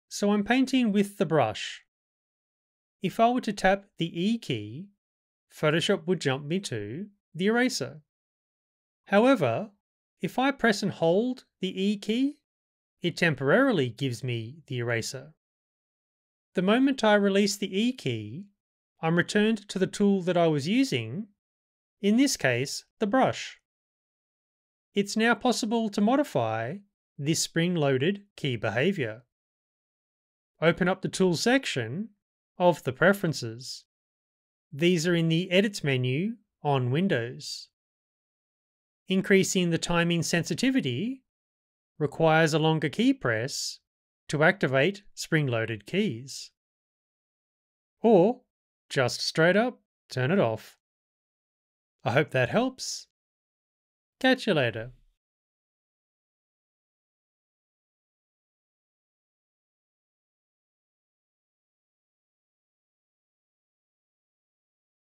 How to Adjust or Remove Spring-Loaded Keys in Photoshop (New Feature June 2022)
Learn how to edit the timing and functionality of spring-loaded keys in Photoshop to temporarily switch between tools. Details below...

0:00 - Spring Loaded Keys in Photoshop

Most Photoshop tools have their own keyboard shortcut. For example...
- Brush Tool: B key
- Eraser Tool: E key

Tapping the assigned key will jump to that tool.

To temporarily switch to a tool, hold down the key that represents that tool. Releasing the key will have Photoshop jump back to whatever tool was just in use. This functionality is referred to as Spring Loaded keys

For example. Tap B to jump to the Brush Tool. Start painting. Press and hold the E key to temporarily switch to the Eraser Tool. Delete some content. Release the E key and Photoshop will switch back to the Brush Tool. Continue painting.

0:24 - Editing Timing of Spring Loaded Keys

To modify the spring-loaded key behaviour, open up the Tools section of the Preferences...

Mac: Photoshop - Preferences - Tools
Windows: Edit - Preferences - Tools

Increasing the Timing Sensitivity requires a longer keypress to activate spring loaded keys.

Uncheck the "Spring-loaded Tool Shortcuts" option to turn off this functionality.

Thanks to @VladimirLadev for the heads up regarding this new features.

Artwork by:
Jeff Hobrath